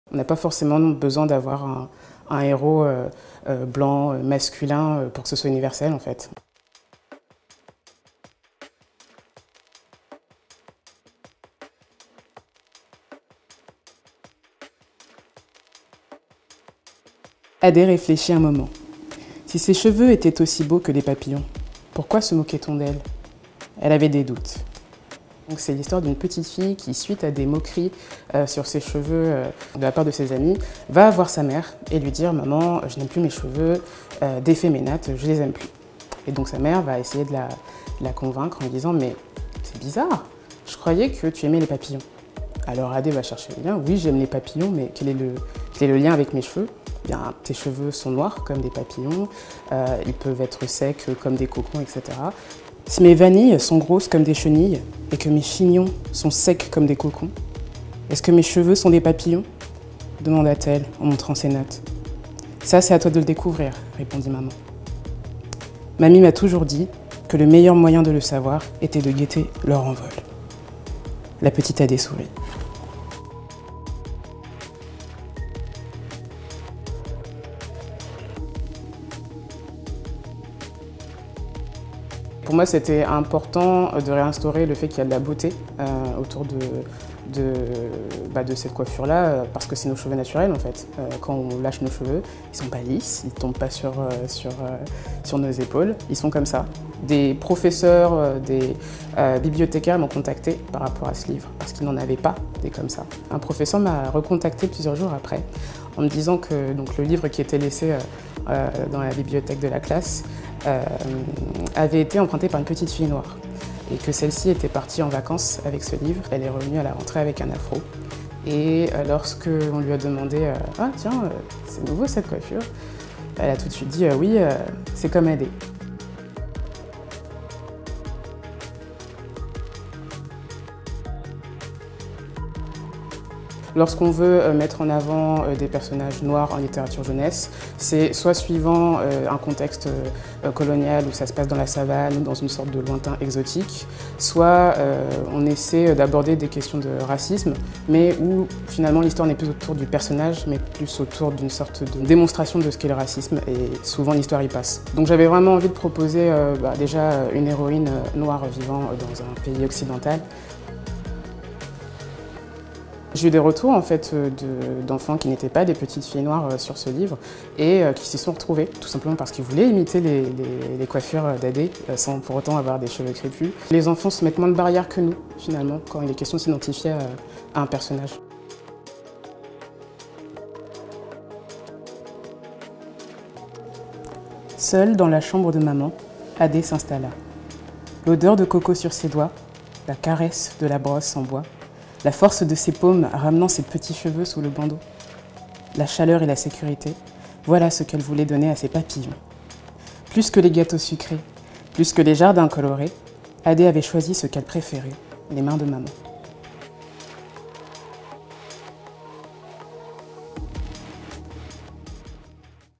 "Comme un Million de Papillons Noirs", un livre d'enfants pour apprendre à aimer son afro
C'est l'histoire d'une petite fille noire qui apprend à aimer ses cheveux crépus. Avec "Comme un Million de Papillons Noirs", Laura Nsafou signe un conte pour enfants plein de tendresse, et prouve au passage que la diversité dans la littérature jeunesse, ça marche.